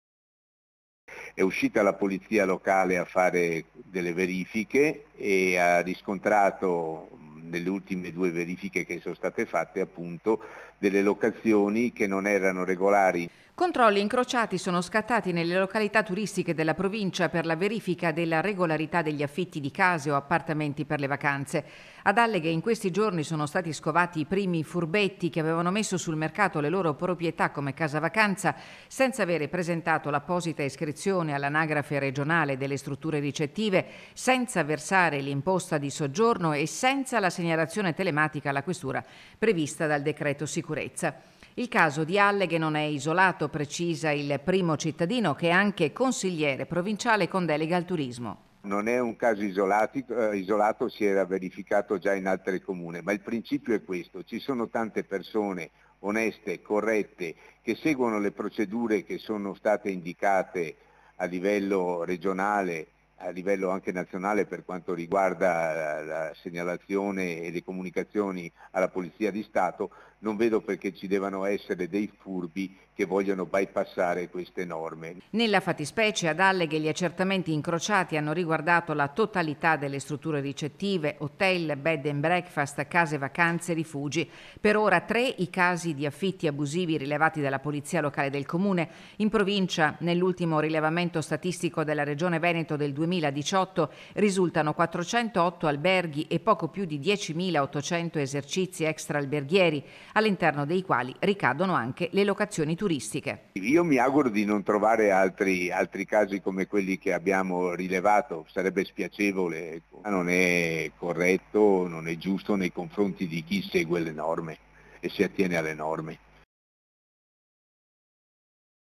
AFFITTI 'FANTASMA', CONTROLLI INCROCIATI CONTRO I 'FURBETTI' DELLE LOCAZIONI TURISTICHE | 21/07/2023
A3 NEWS Veneto 21/07/2023 - ALLEGHE - Accertamenti sulle locazioni turistiche "fantasma". Già 3 i casi accertati ad Alleghe. Il sindaco Danilo De Toni, consigliere provinciale con delega al turismo: "Un danno per tutti, per il turismo e per chi rispetta le regole".  || Controlli incrociati sono scattati nelle località turistiche della provincia per la verifica della regolarità degli affitti di case o oppartamenti per le vacanze. Ad Alleghe in questi giorni sono stati scovati i primi "furbetti" che avevano messo sul mercato le loro proprietà come casa vacanza senza aver presentato l'apposita iscrizione all'anagrafe regionale delle strutture ricettive, senza versare l'imposta di soggiorno e senza la segnalazione telematica alla Questura prevista dal Decreto Sicurezza. Il caso di Alleghe non è isolato - precisa il primo cittadino di Alleghe che è anche consigliere provinciale con delega al turismo.Nella fattispecie ad Alleghe gli accertamenti incrociati hanno riguardato la totalità delle strutture ricettive, hotel, bed and breakfast, case vacanze, rifugi. Per ora 3 i casi di affitti abusivi rilevati dalla polizia locale del Comune. In provincia, nell'ultimo rilevamento statistico della Regione Veneto del 2018, risultano 408 alberghi e poco più di 10.800 esercizi extralberghieri, all'interno dei quali ricadono anche le locazioni turistiche. - Intervistati DANILO DE TONI SINDACO DI ALLEGHE (AL TELEFONO) (Servizio di Tiziana Bolognani) - Segui Antenna Tre anche sul digitale terrestre! Visita il sito